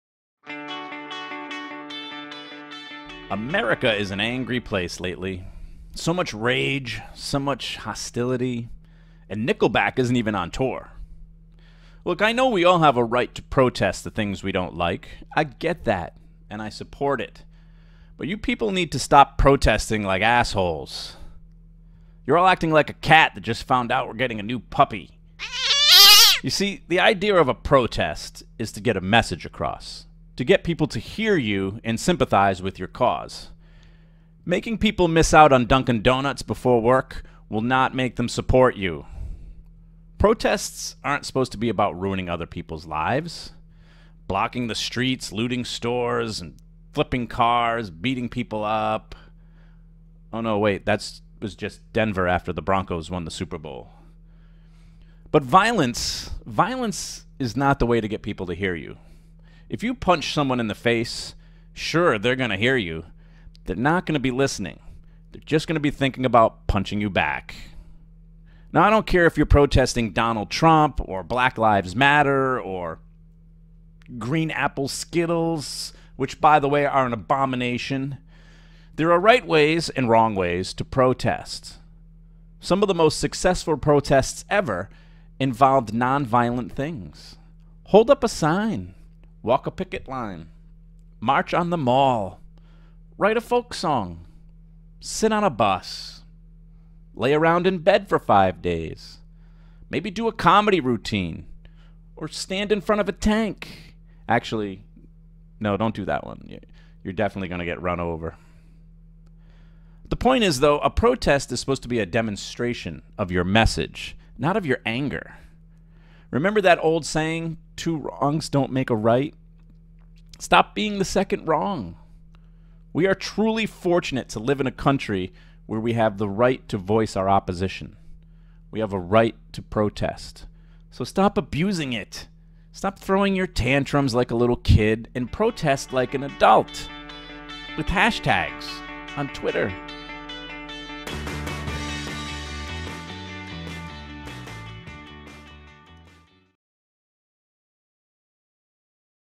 Your Protests Suck! - Tom Stewart Project
Your Protests Suck!
Stop Using Violence - Donald Trump - Black Lives Matter
Tom Stewart Project

Hosted, Written, and Edited by:
Comedian Tom Stewart